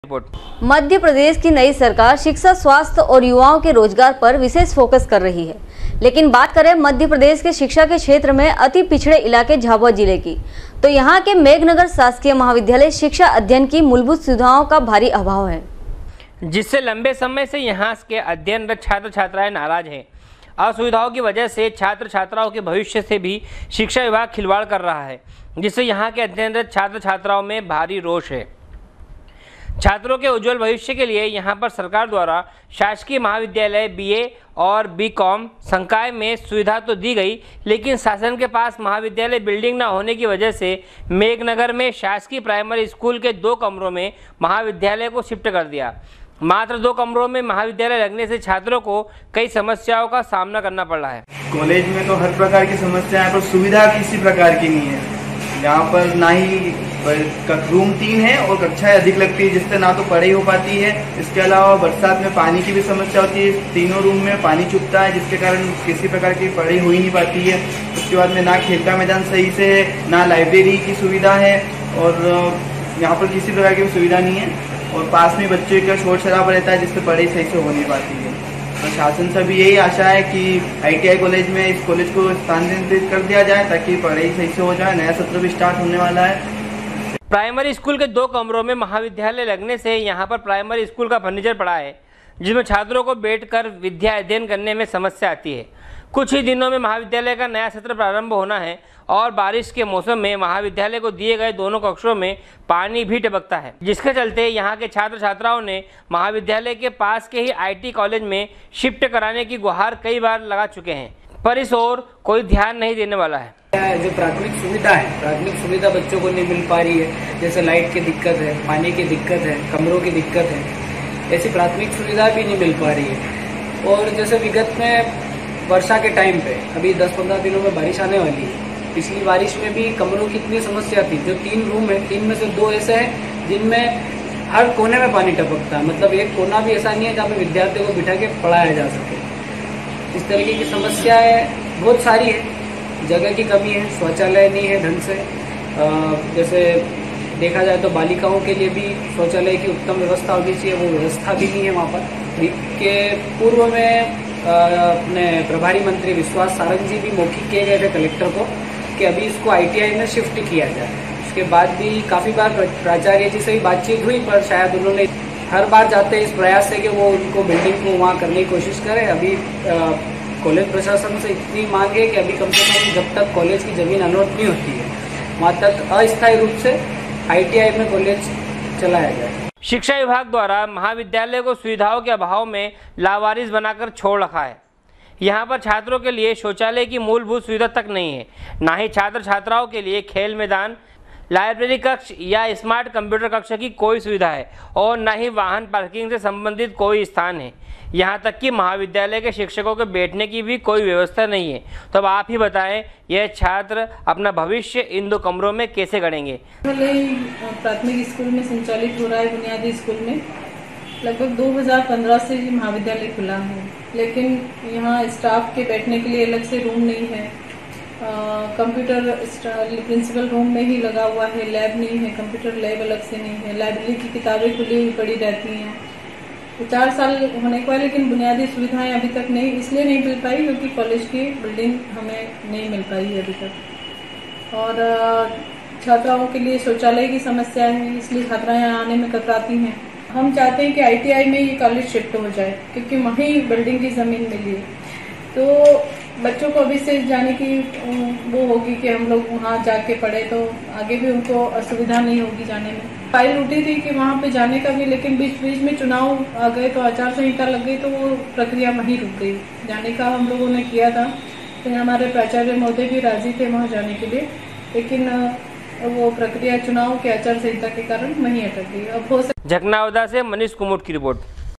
sw24news zhaknawada, 09.06.2019
मध्य प्रदेश की नई सरकार शिक्षा स्वास्थ्य और युवाओं के रोजगार पर विशेष फोकस कर रही है। लेकिन बात करे मध्य प्रदेश के शिक्षा के क्षेत्र में अति पिछड़े इलाके झाबुआ जिले की तो यहां के मेघनगर शासकीय महाविद्यालय शिक्षा अध्ययन की मूलभूत सुविधाओं का भारी अभाव है